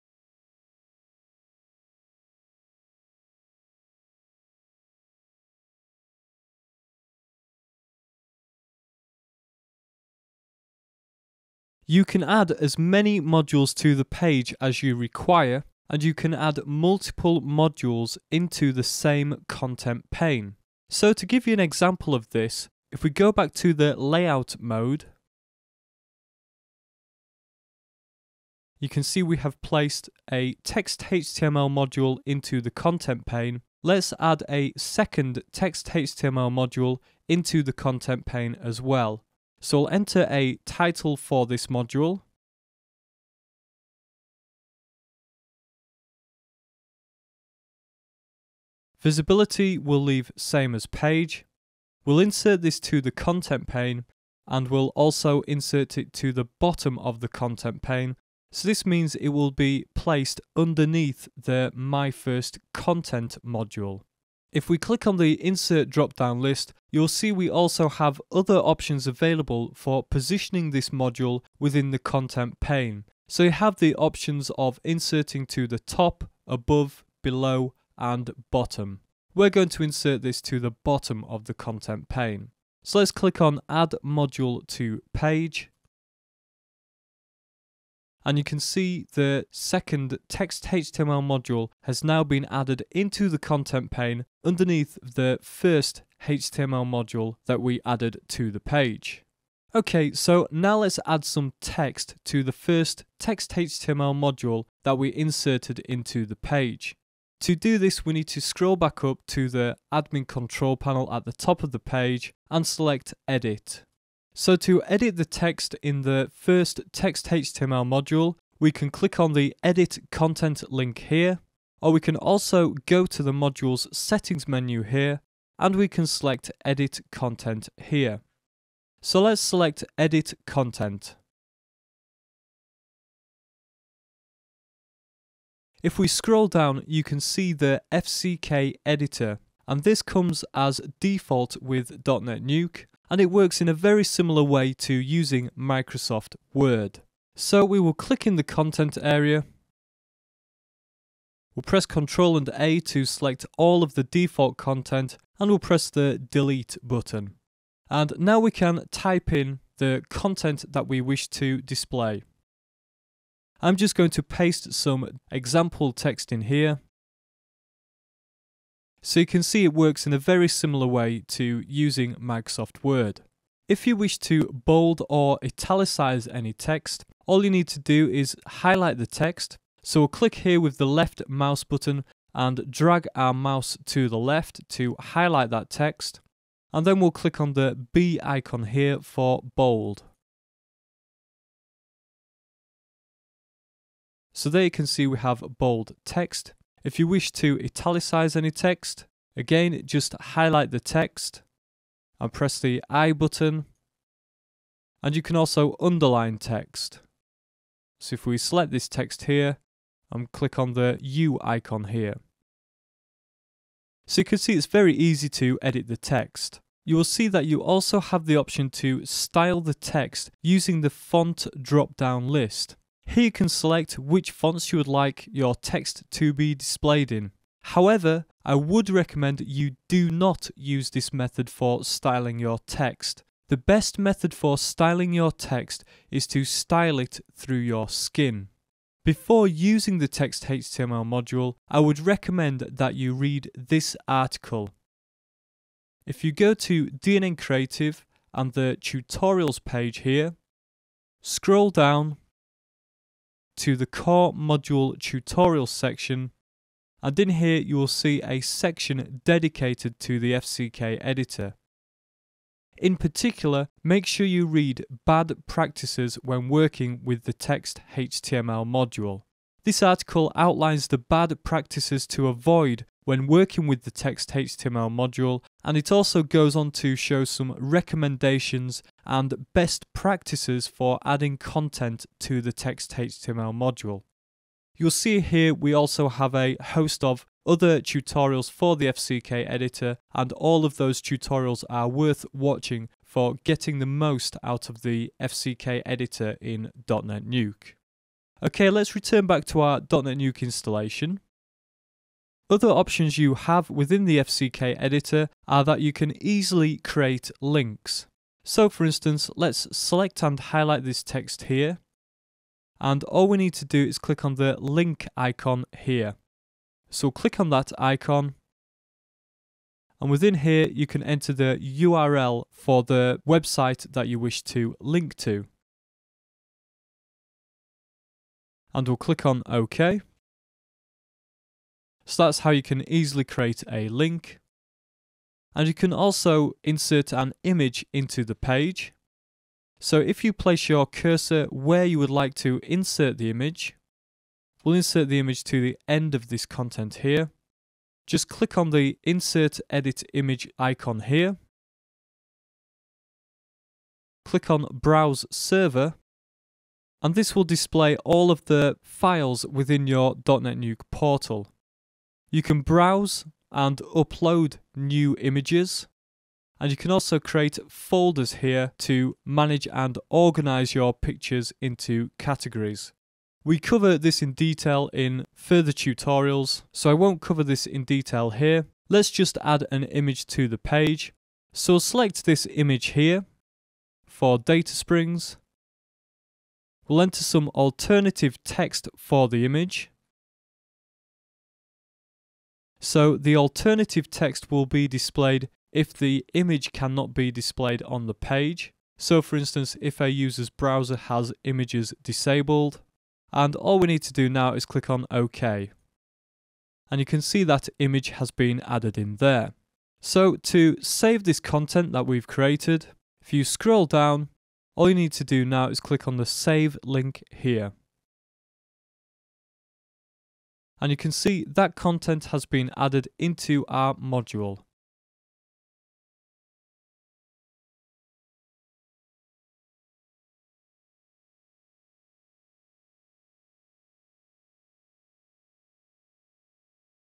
DotNetNuke 5 Quick Start Guide - Video 3 of 6
How to Add a Text / HTML Module to the Page

This tutorial will help you get started using DotNetNuke without getting into all of the finer details, once you've got to grips with this tutorial you can then explore the other video tutorials on DNN Creative Magazine and those tutorials will help you to get the most from your DotNetNuke portal.

How to add multiple modules into the Content Pane

How to edit the content of a Text / HTML module

How to use the FCK Editor for adding text content

How to Bold, Italicise, Underline the text

How to create a link

How to add an image